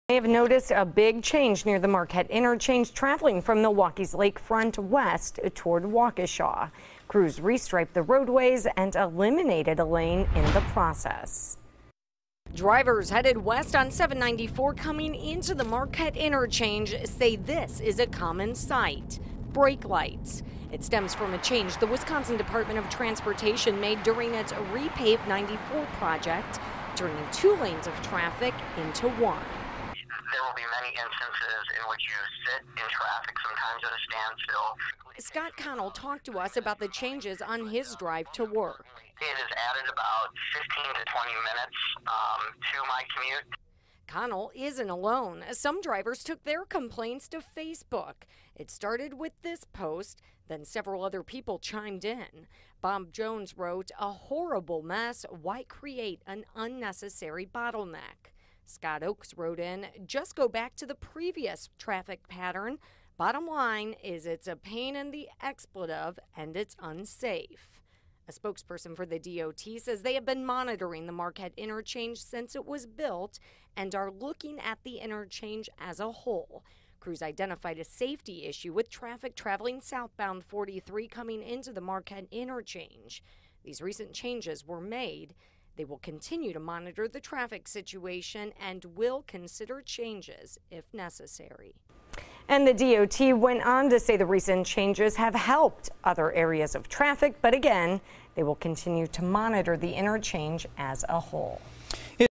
I-94 westbound lane eliminated downtown in re-configuration
Melissa McCrady reports.